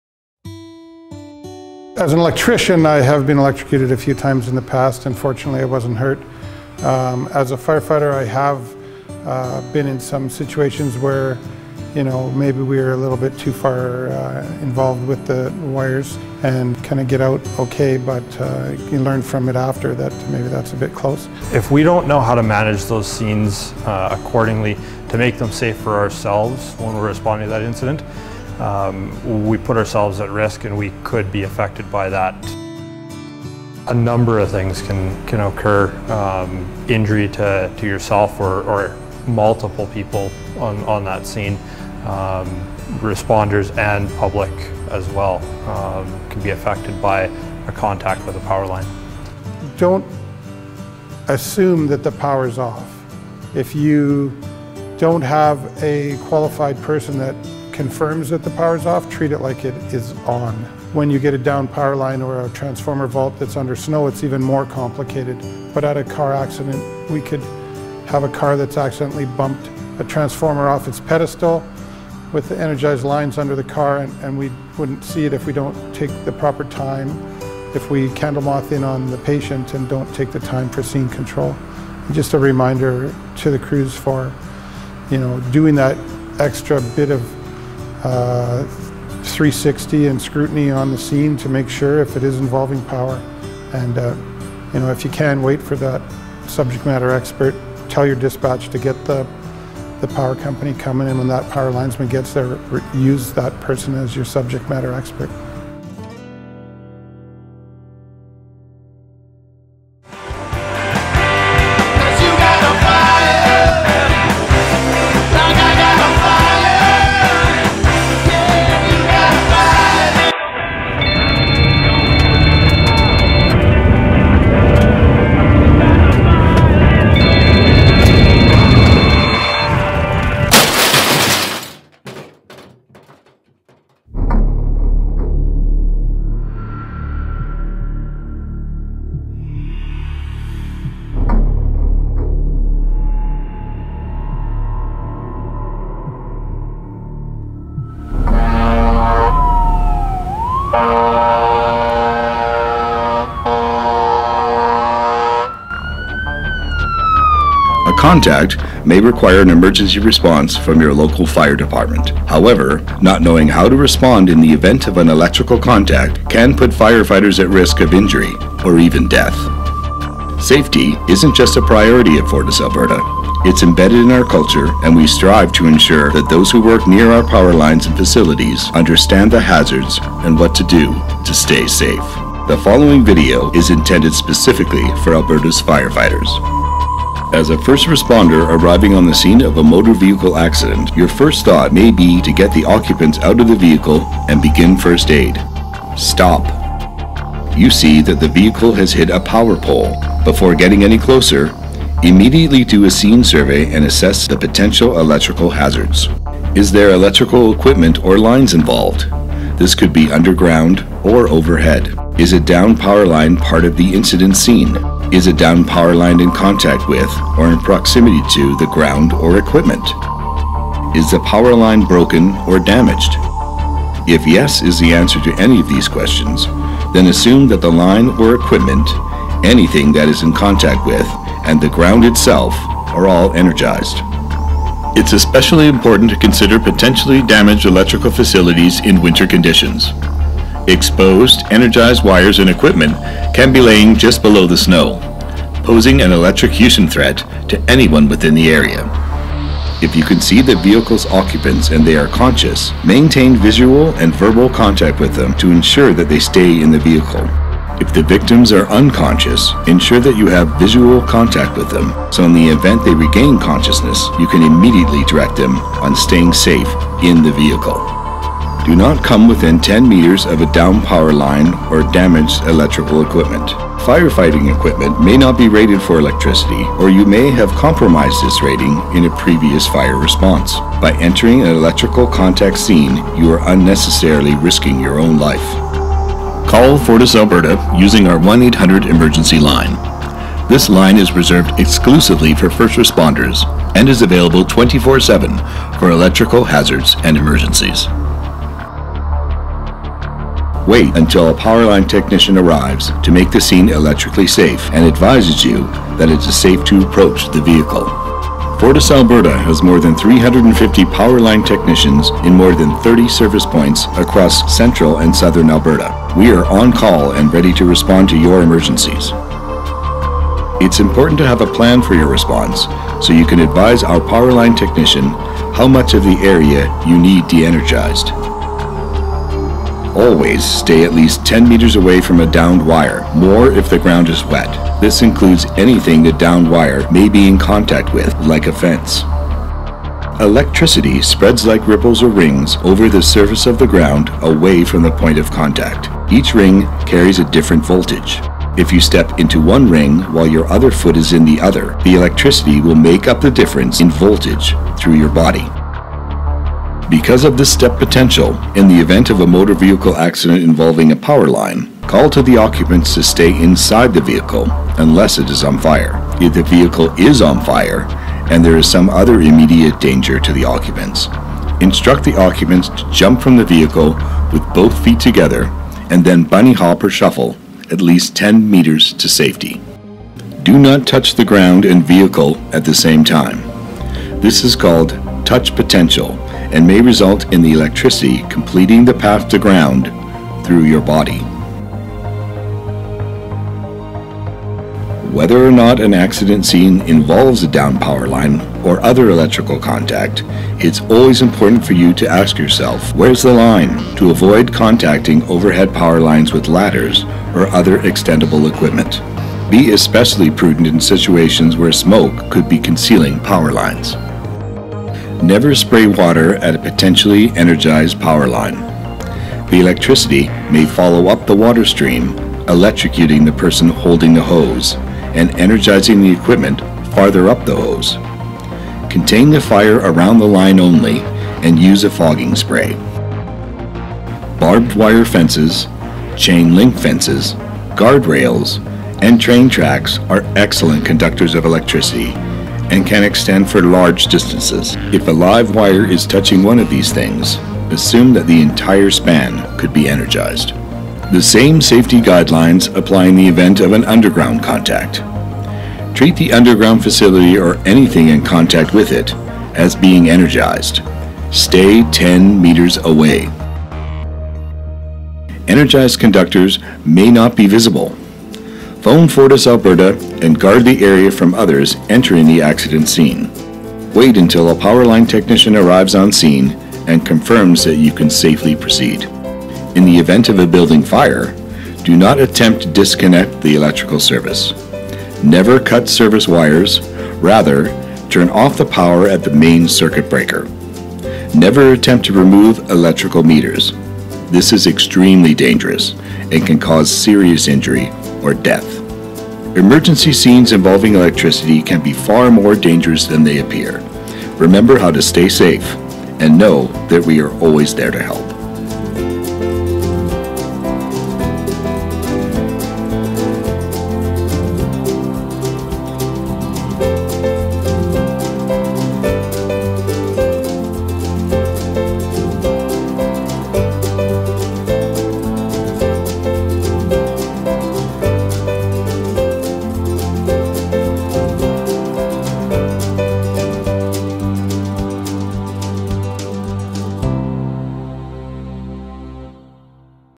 Power Line Safety for Firefighters YouTube
This video was created for Alberta Firefighters to enhance awareness and safety when responding to emergency scenes involving power lines and other electrical equipment.